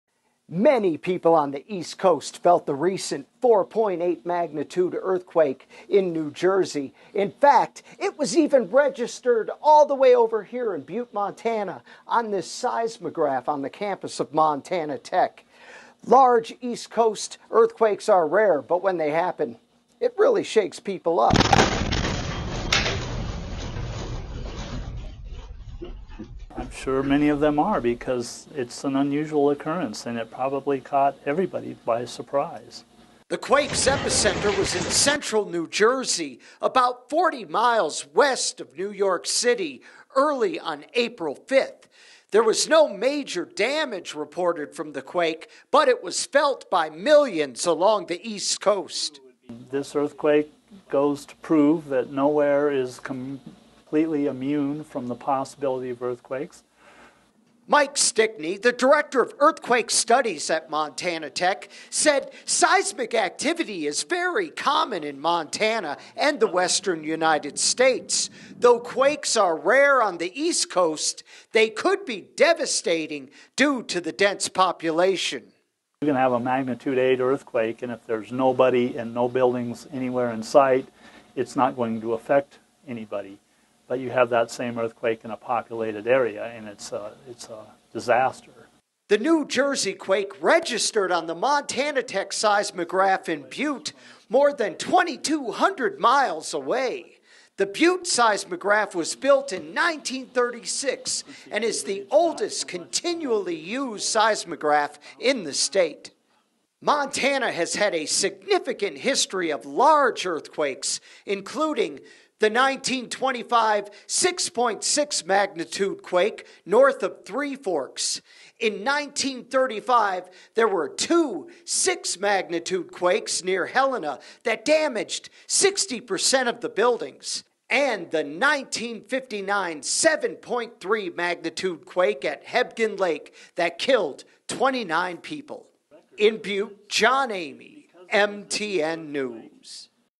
Butte seismograph registered New Jersey earthquake from 2,200 miles away
The Butte seismograph was built in 1936 and is the oldest continually used seismograph in the state.